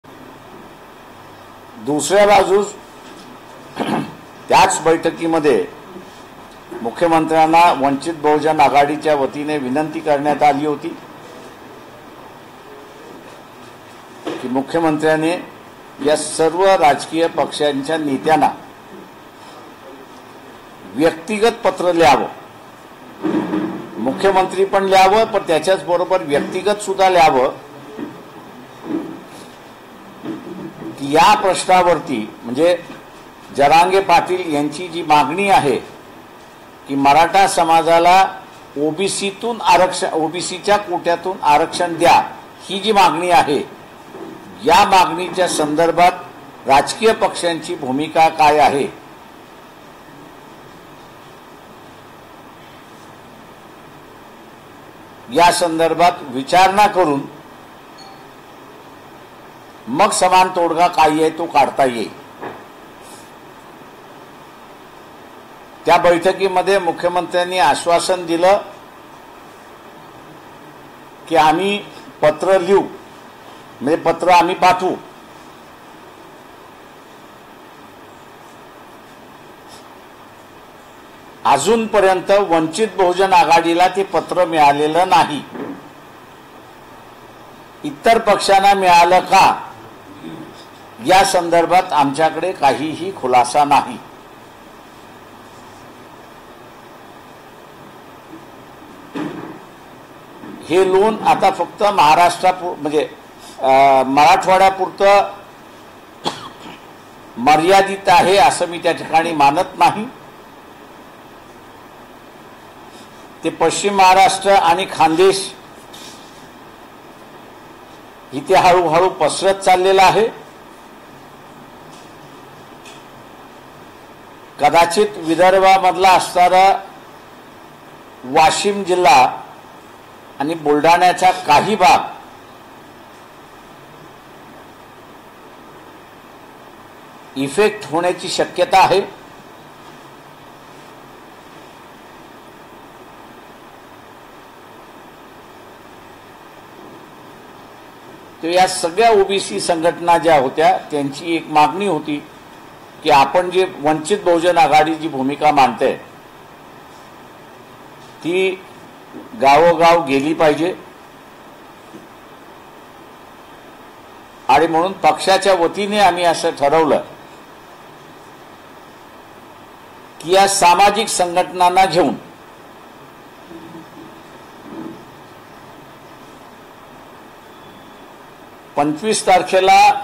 Prakash Ambedkar Manoj Jarange। जरांगेंच्या मागणीवर राजकीय पक्षांची भूमिका काय? - प्रकाश आंबेडकर
Prakash Ambedkar Live | वंचित प्रकाश आंबेडकर काय मोठी घोषणा करणार? पत्रकार परिषद लाईव्ह । Zee 24Taas